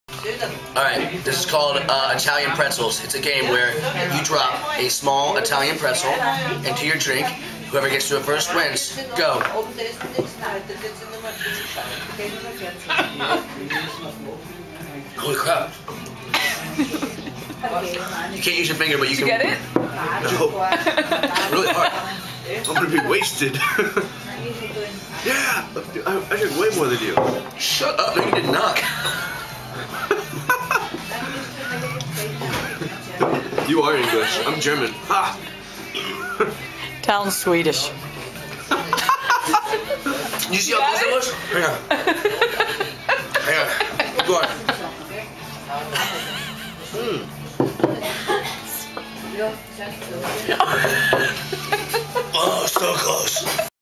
TALON REID'S ITALIAN PRETZEL DIVING PART 1
TALON REID AND TY JAMES DIVE FOR PRETZELS IN ITALY AT 11AM LOL